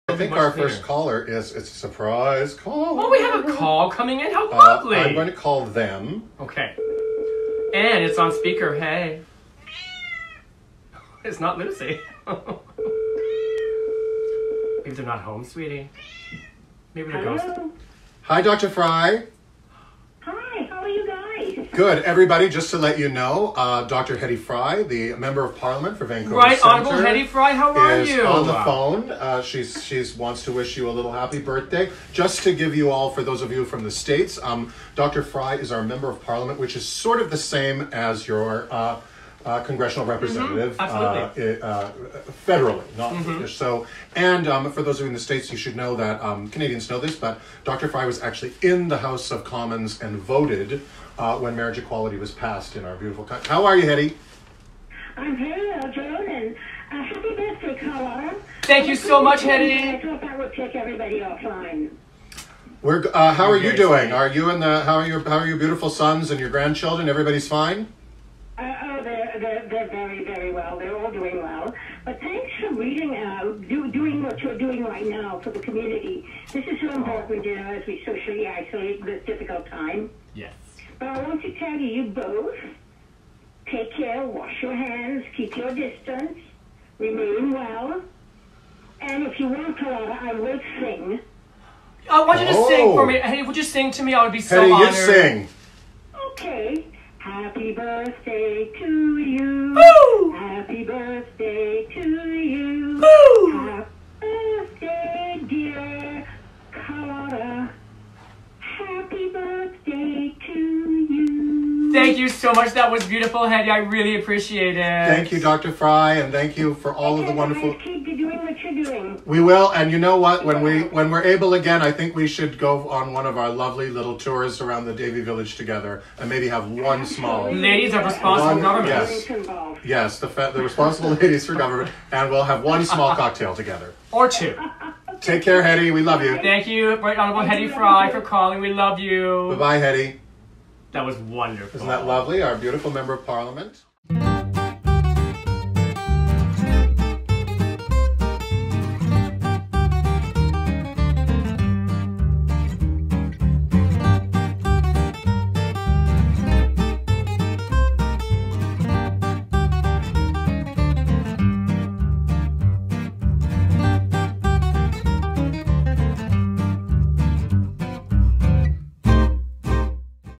Quarantinily Yours with Carlotta Gurl & Joan-E
I spent Saturday night, virtually with two gorgeous 6’ men!  Okay, 6’3” in heels.

Stay home. Wash your hands.

Happy birthday Carlotta Gurl!